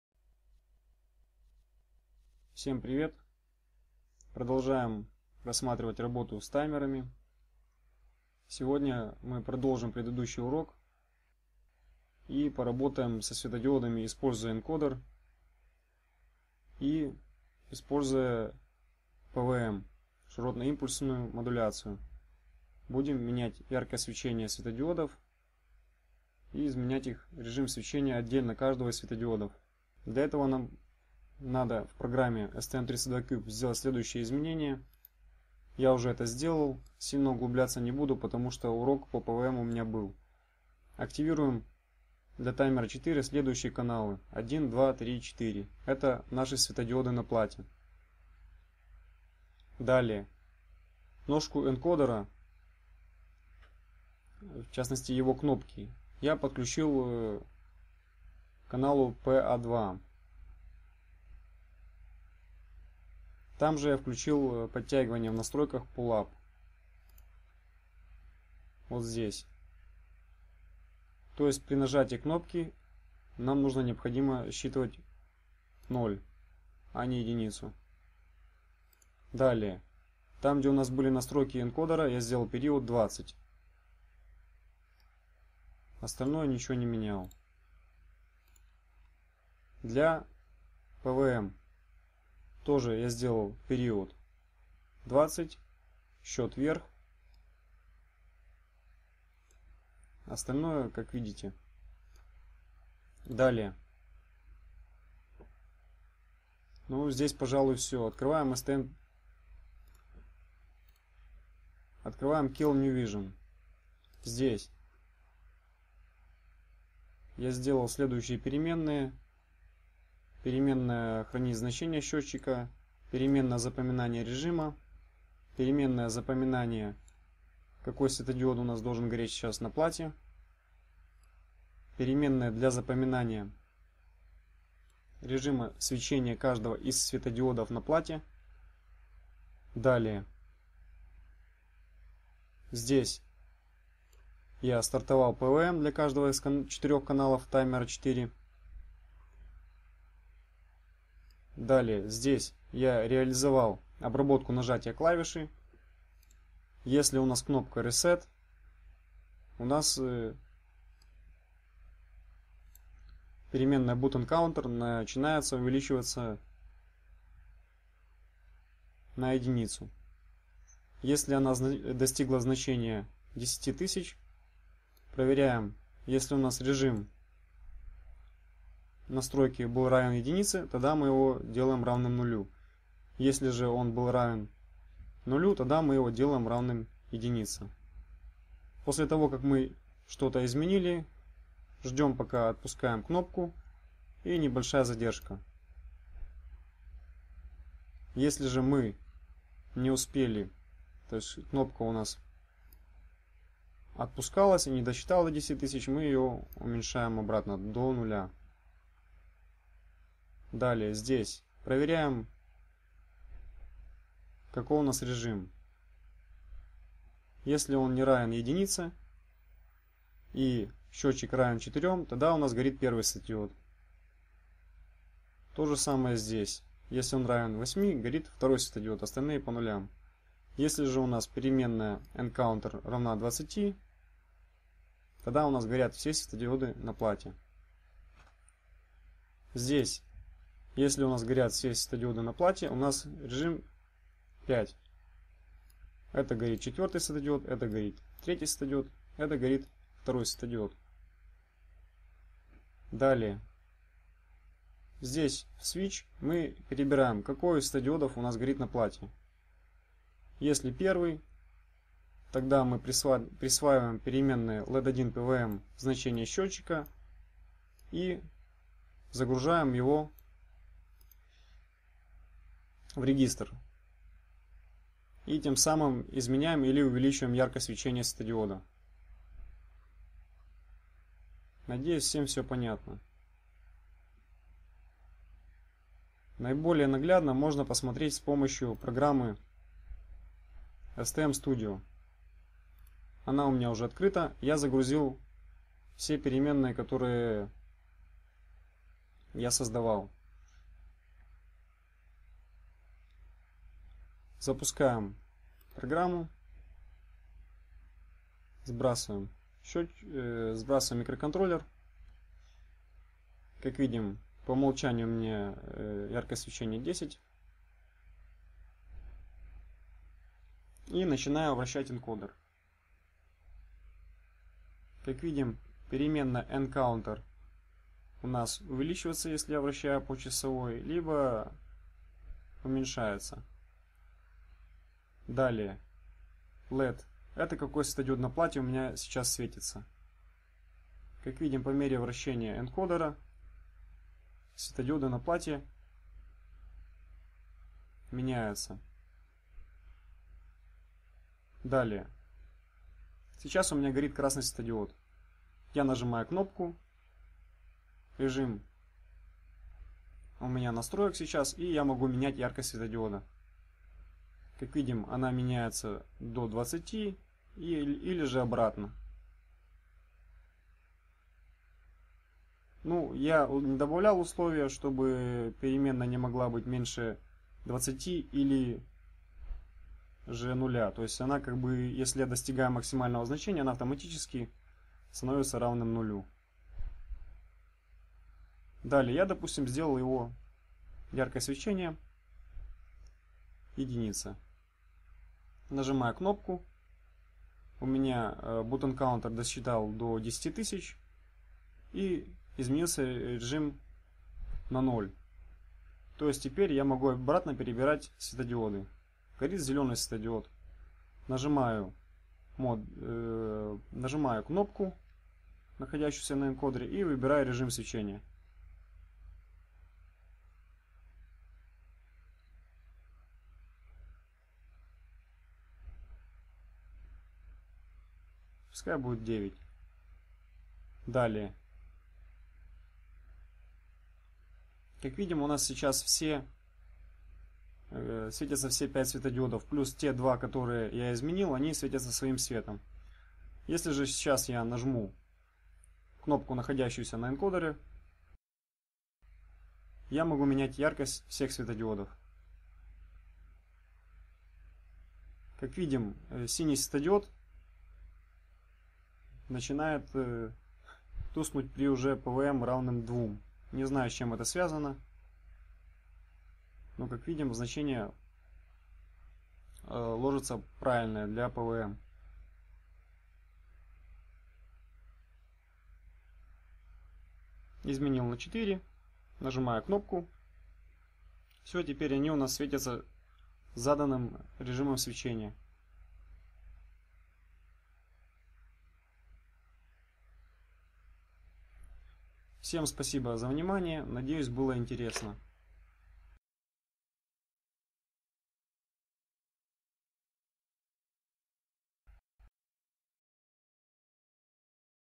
STM32Cube Encoder +PWM (СТМ32 энкодер + ШИМ)
STM32discovery - регулировка яркости свечения светодиодов используя PWM и энкодер.